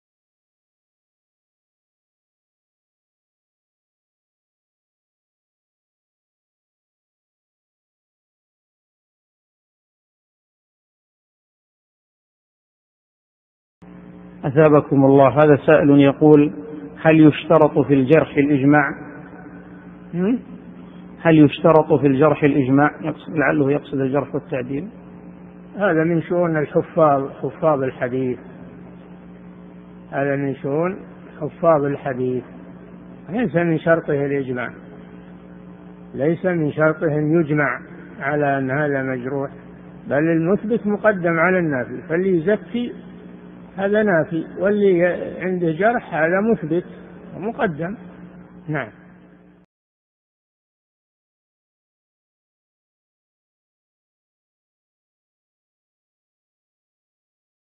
A duhet pasur konsensus për të kritikuar një individ?
Imam Salih bin Feuzan el-Feuzan
Data e fetvas: 18/8-1430 ose 9/8-2009
Përktheu: Valdet Gashi

Pyetje: A kërkohet konsensus për ta bërë xherh (kritikë) dikë? Pyetësi sigurisht ka për qëllim Xherh ue Ta’dil (kritikën dhe lavdërimin).

Përgjigje: Kjo i përket çështjeve të studiuesve të Hadithit. Konsensusi për këtë lloj kritike nuk është kusht. Nuk është kusht që ata të pajtohen se një person duhet të kritikohet. Ai që pohon kritikën, ka përparësi para atij që mohon kritikën. Ai që lavdëron mohon dhe ai që kritikon pohon, dhe prandaj ai ka përparësi.